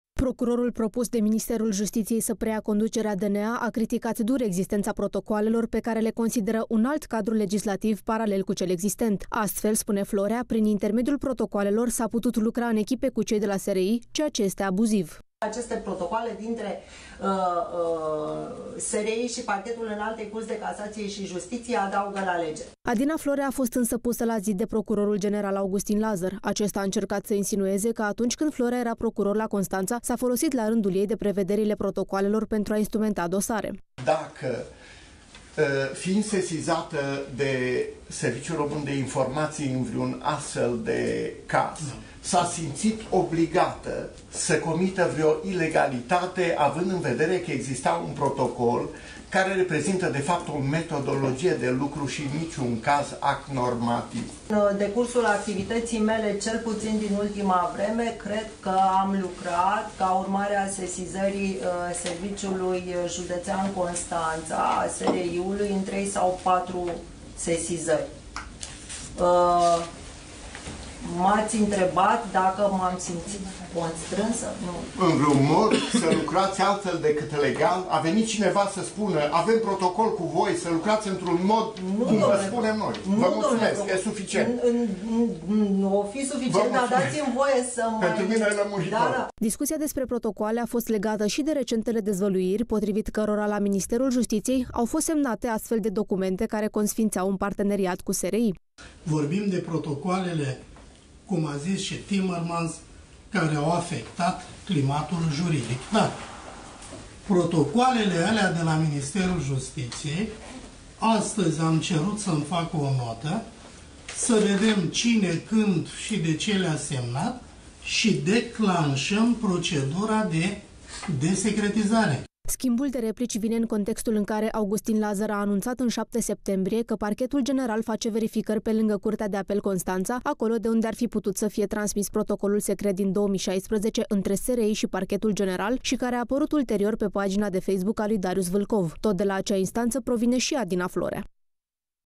Scandal uriaş în CSM din cauza protocoalelor SRI-Parchetul General
Protocoalele SRI-Parchetul General au provocat un scandal uriaş în CSM. Augustin Lazăr, Adina Florea şi ministrul Tudorel Toader s-au contrat pe utilitatea documentelor semnate între cele două instituţii şi pe cât de dăunătoare au fost Justiţiei. Cum era de aşteptat, cel mai mare apărător al protocoalelor a fost procurorul General, care a încercat să inducă ideea că au fost doar chestiuni tehnice, nicidecum nu au adăugat la lege, aşa cum a susţinut Adina Florea.